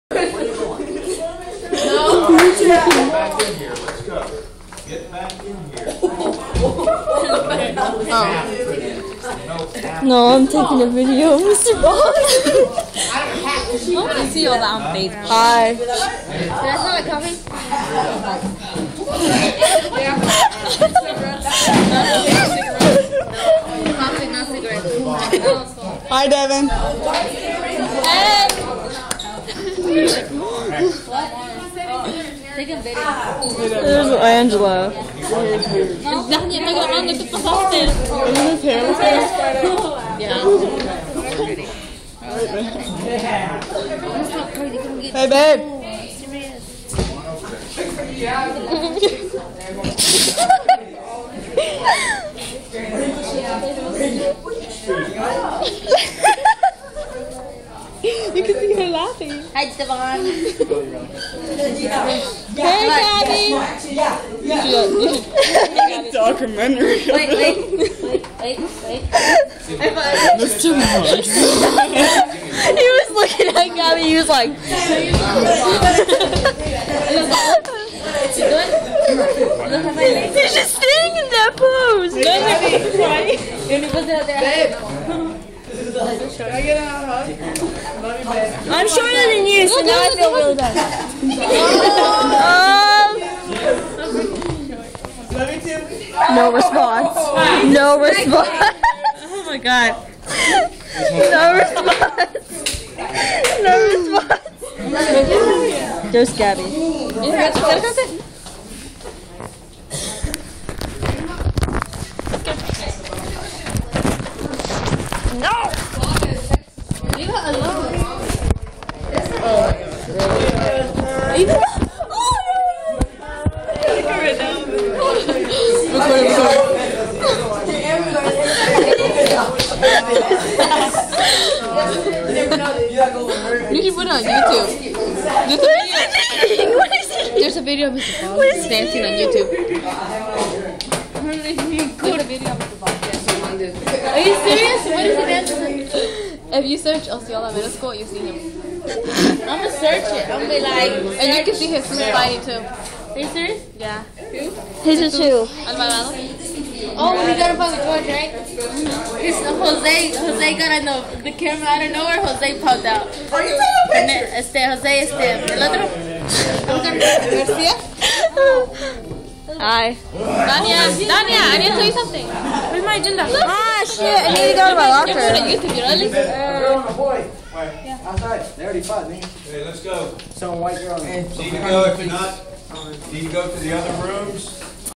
My Classroom
We live in Okeechobee. This is my Middle School! (Osceola Middle School) my teachers name is Mr. Boggs. :) hehe hope you like our craziness ;)